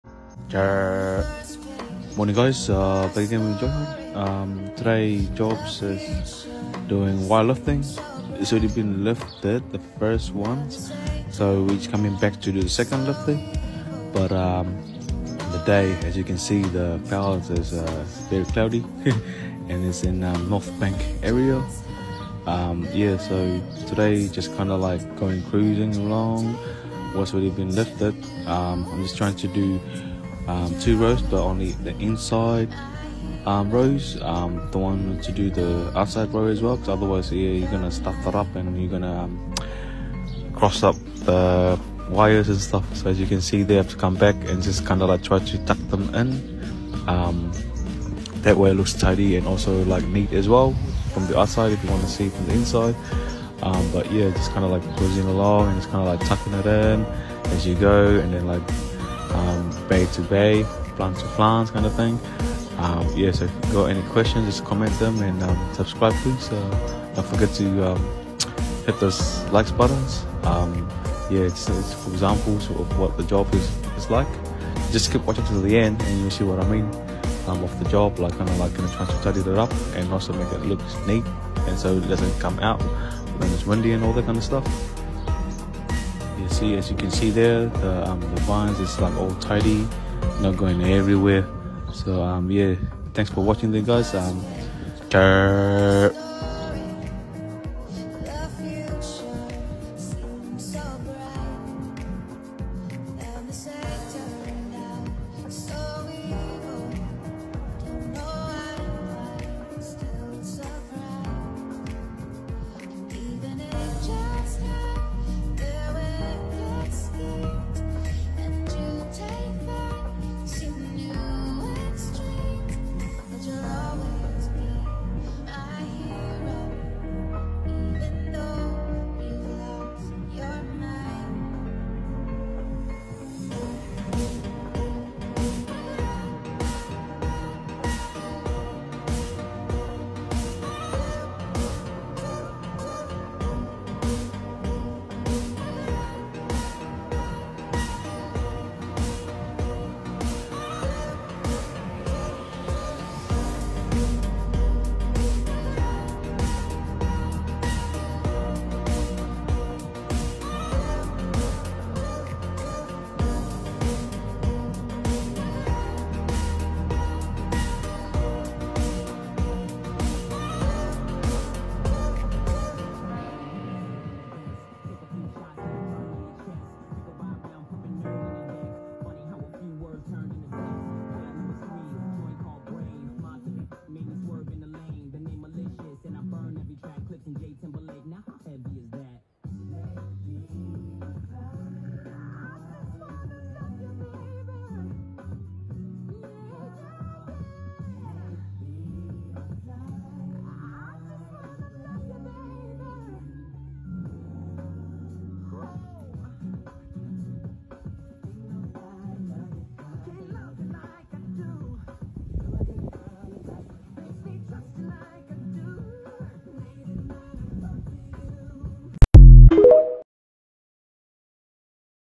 Wire lifting on a cloudy day
Wirelifting job on a cloudy day 😂Inilah pekerjaan di VINEYARD blenheim Marlborough region New Zealand AOTEAROA 🇳🇿 Fruit thinning